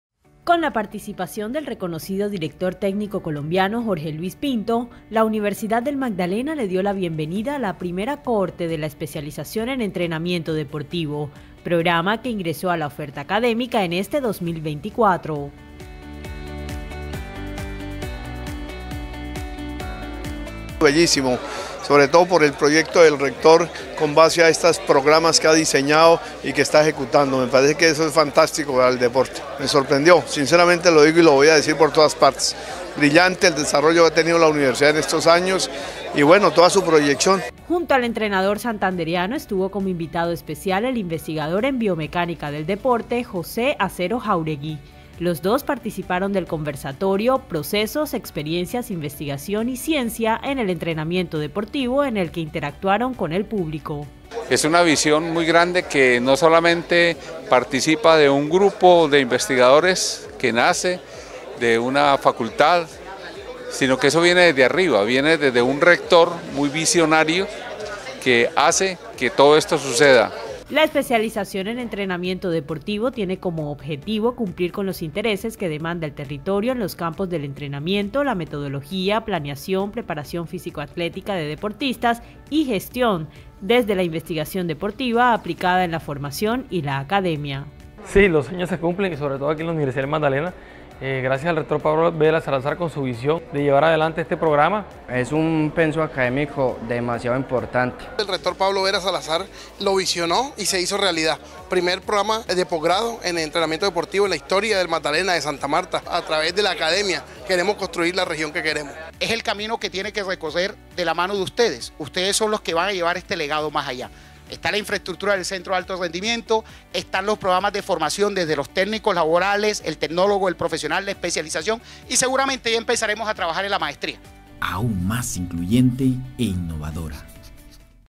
Con grandes invitados inició el camino académico de la Especialización en Entrenamiento Deportivo
La Universidad del Magdalena, a través del Centro de Posgrados y Formación Continua, realizó el lanzamiento de la primera cohorte de la Especialización en Entrenamiento Deportivo; un grupo de estudiantes que eligieron la alta calidad de nuestra Alma Mater para cualificar su formación profesional. A ellos, los recibimos en compañía de dos invitados especiales que viven día a día del deporte y con quienes compartieron experiencias y conocimiento.